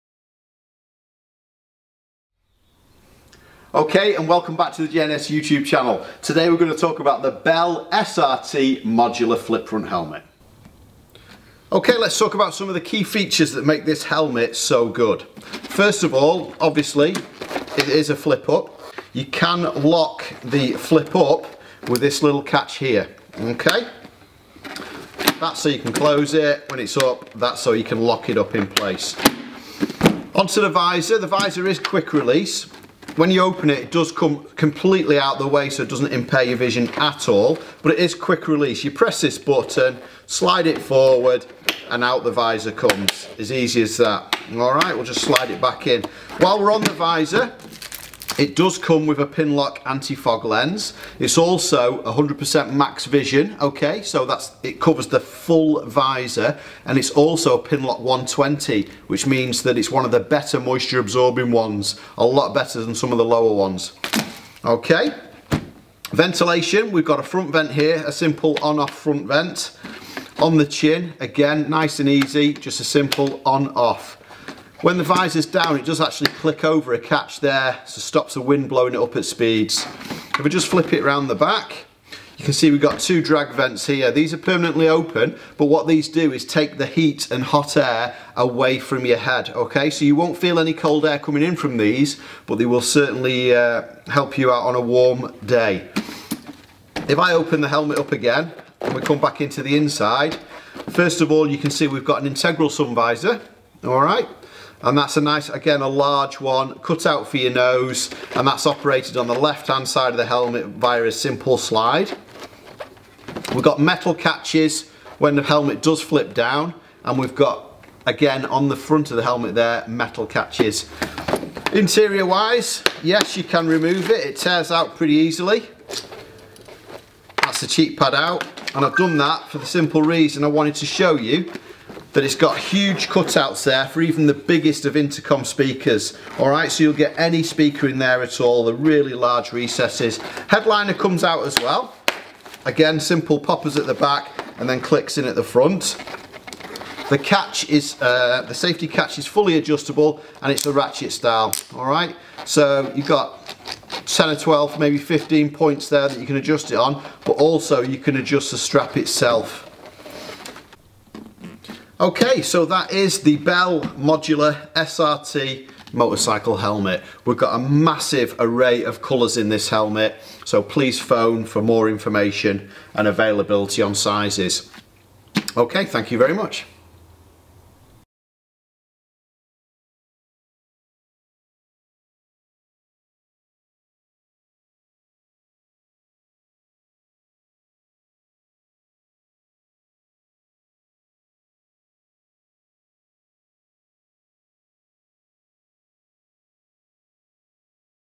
Bell SRT Modular Helmet - J&S Accessories Ltd
The Bell SRT Modular White flip-front motorcycle helmet is a premium motorcycle helmet constructed from ultra-lightweight yet highly durable Fibreglass. The Bell SRT also comes with five-year warranty from Bell. The SRT has an anti-scratch Panovision visor with class one optics for a wider vision field. This also has glasses grooves for the glasses wearer as well as recapped EPS pockets for blue tooth compatibility. The interior is removable and washable and this is ECE approved.